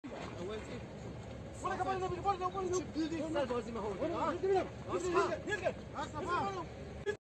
ضرب و شتم یک کولبر در نوارمرزی نودشه
امروز یکشنبه ۱۶ مردادماه ۱٤٠١، نیروهای هنگ‌مرزی مستقر در ایست بازرسی پاسگاه ملندو مرز نودشه یک کولبر را مورد ضرب و شتم قرار دادند.
نیروهای هنگ‌مرزی این کولبر را بخاطر حمل دو گالن بیست لیتر بنزین مورد ضرب و شتم قرار داده‌اند.